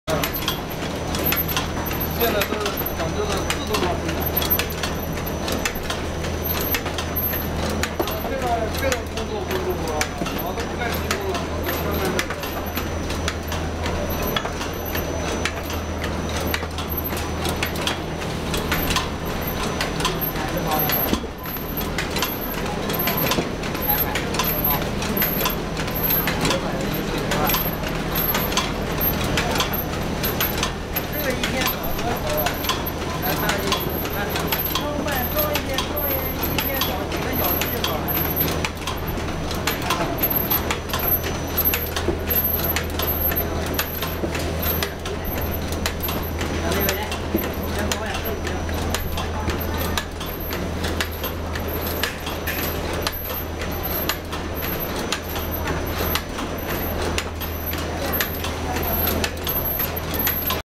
Cigarette Box Three-dimensional transparent film packing machine Cellophane Overwrapping Packer
Cigarette Box Wrapping Machine is widely applicable to packaging various single large boxed articles with a gold tear tape for easy opening, such as medicine, health products, foodstuff, cosmetics, stationery and audiovisual products, or collective film wrapping of multi-piece boxes.

Performance Characteristics:

1. PLC control and touch screen operation, simple and easy.
2. Uses inverter to control the speed, so could adjust according to the operation need.
3. Could connect with another machine to make a whole production line, save labor cost greatly.
4. The mould change easy, without dismantle drive chain, hopper. Save time greatly.
Packaging material
BOPP film,heat seal shrink film
Packaging speed
30-100 parcels/min
Packaging sizemm
length60-130 width40-100 thickness10-40
Power source
220V 50 HZ
Power
3 KW
working atmospheric pressure
0.5 Mpa
Whole machine weight
about750 Kg
Appearance size
2700*700*1450 mm